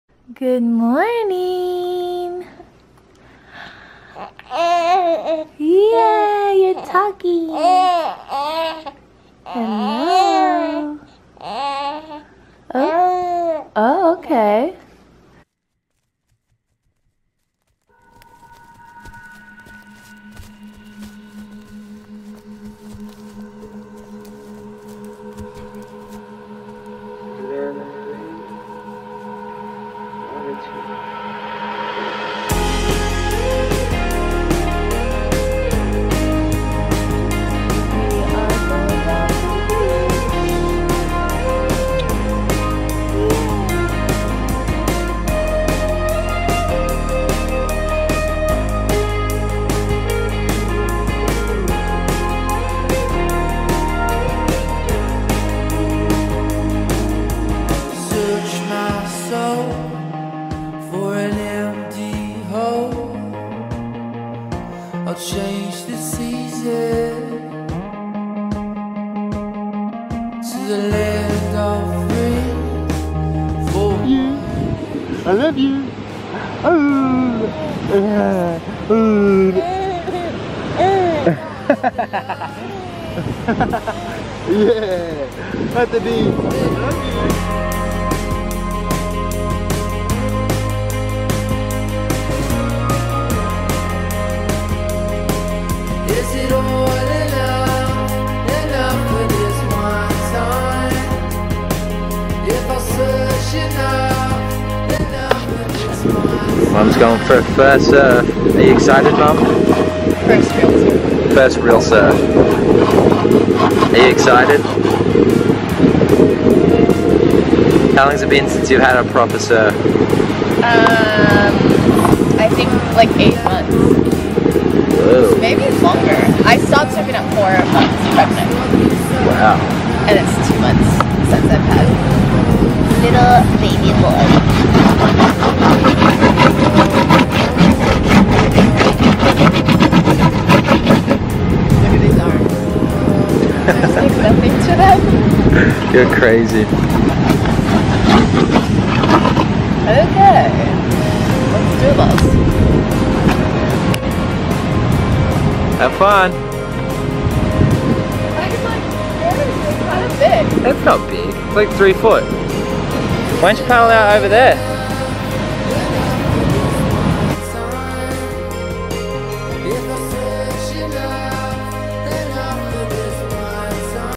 Family beach day on Kauai
Here is our last day on kauai. I got to go for a surf but didn't catch to many waves.